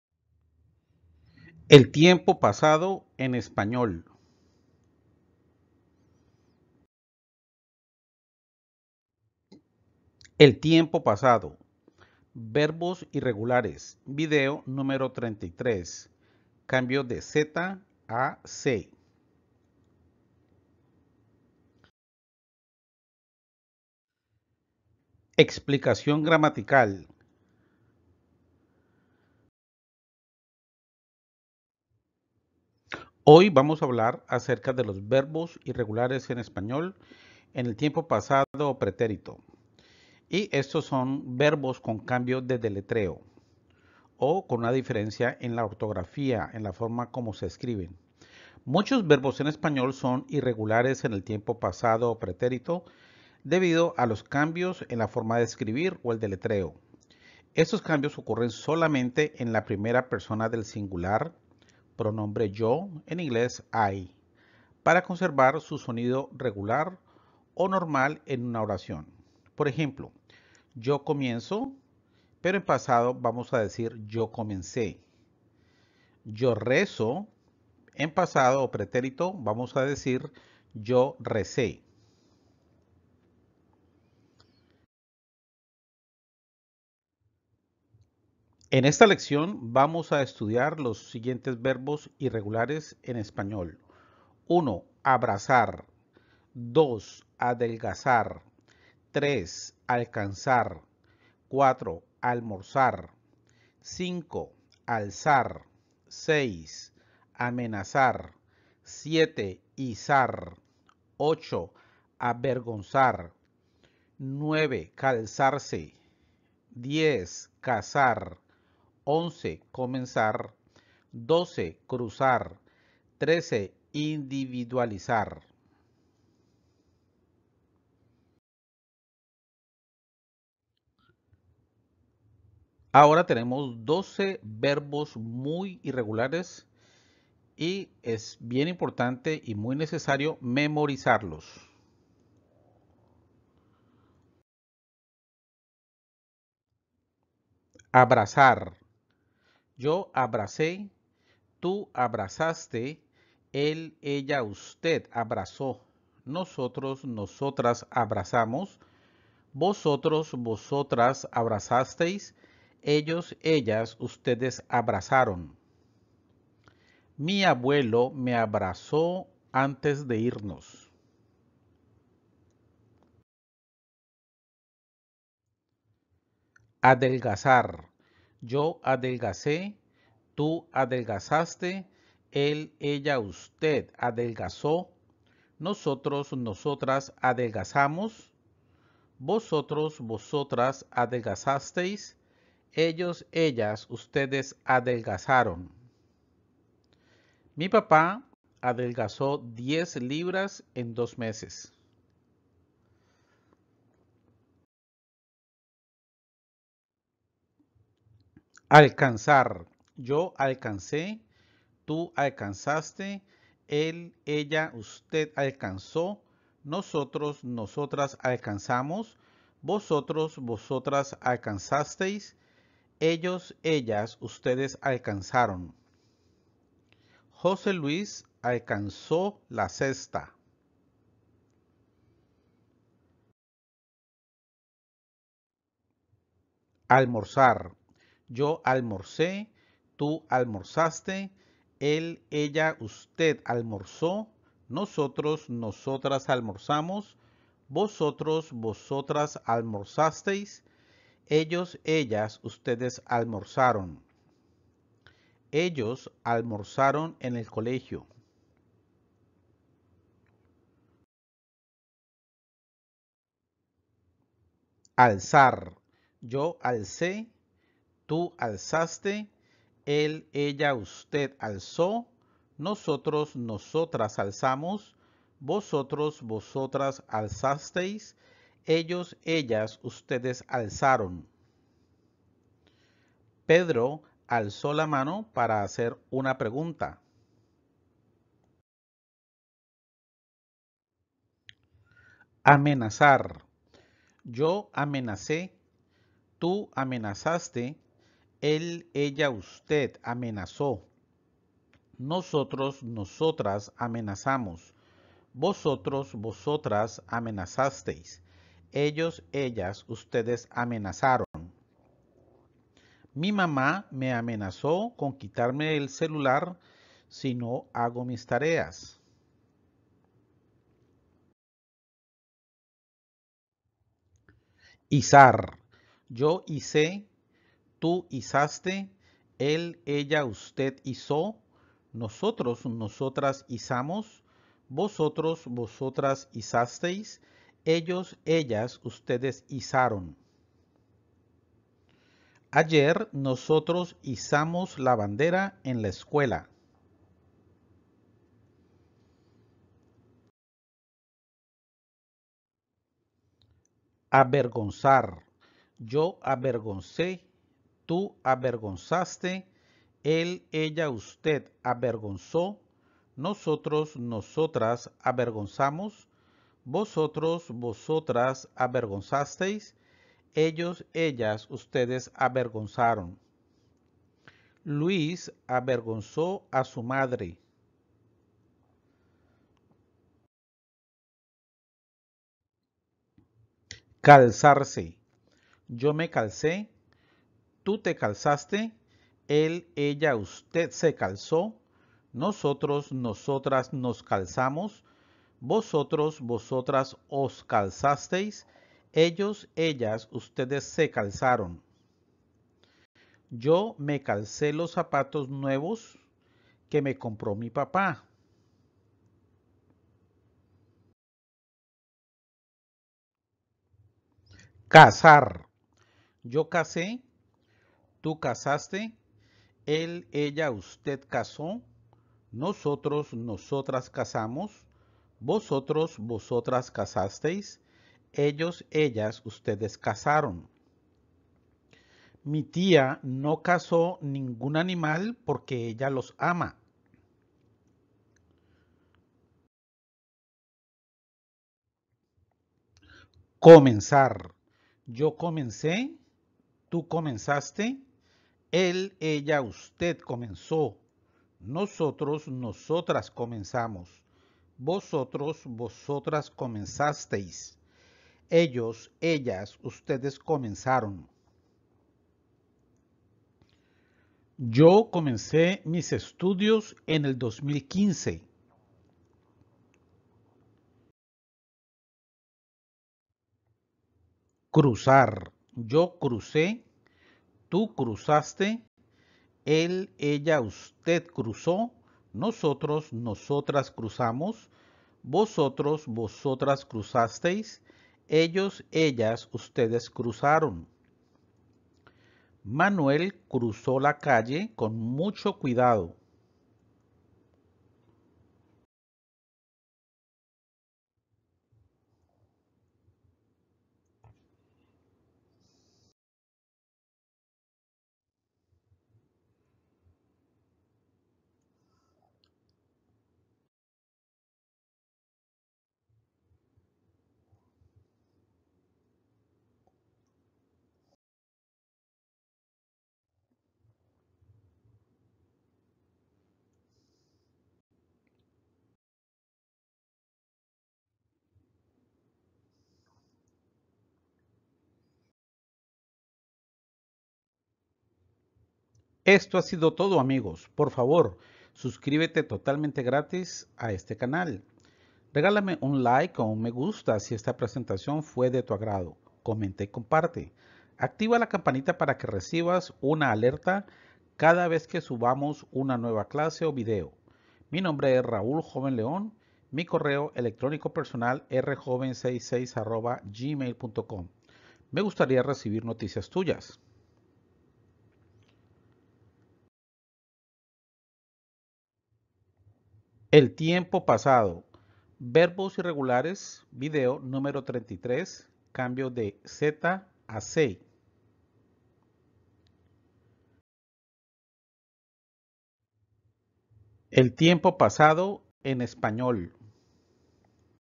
#33. El tiempo Pasado o Pretérito en español. Verbos Irregulares cambio de /Z/ - /C/
Hoy vamos a estudiar un tiempo verbal muy útil y de gran importancia en español: El Tiempo Pasado o Pretérito.  Este es el video #33 de esta lista de reproducción dedicada a este tiempo de los verbos irregulares con cambio de raíz en la primera persona del singular (yo = I) de la letra /Z/ a /C/

CANAL DE ESPAÑOL
 Magicalspanish (Youtube)
 @mymagicalspanish (Facebook)
 @JovenLen (Twitter)
 @ingleswithraul (Facebook)
 @inglés_joven (Twitter)